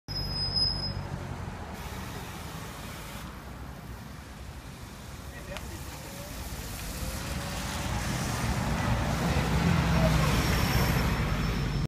【スロバキア】 ブラチスラヴァの連節バス Articulated bus of Bratislava, Slovakia (2014.4)
ブラチスラヴァ中央駅前の連節バスです。
2014年4月、iPhone5sで撮影。

Articulated bus of Bratislava Central Station square.

スロヴァキア　ブラチスラバ　連接バス
Slovenská
Bratislava hlavná stanica
DPB, Dopravný podnik Bratislava
BID, Bratislavská integrovaná doprava
4143 IMG 5187